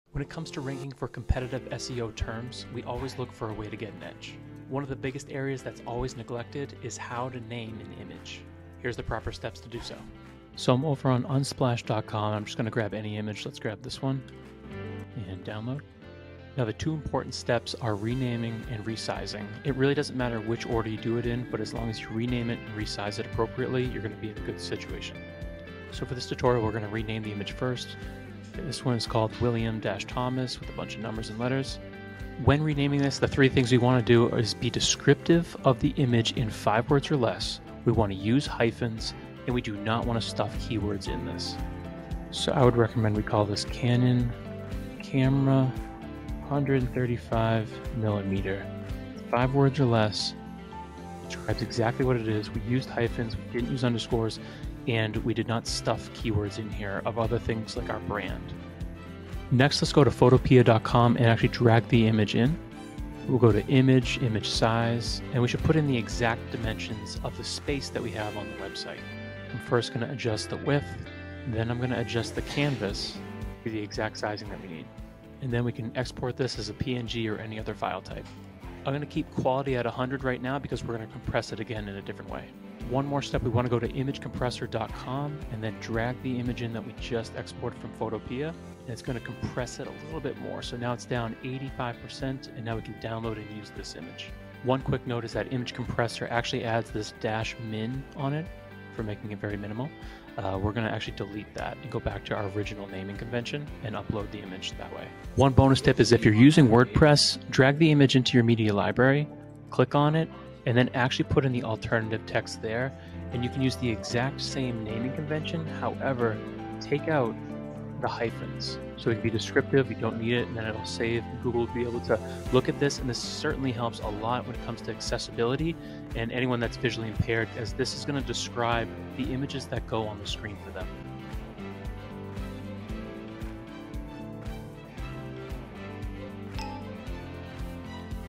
How to Name Images for SEO Ranking
This tutorial shows exactly how to name images for SEO.

We understand the significance of optimizing images to achieve better rankings on Google and other search engines. In this comprehensive tutorial, Matt will walk you through the best practices for naming images, compressing and resizing, adding alt text and titles, using structured data, and monitoring your image SEO performance.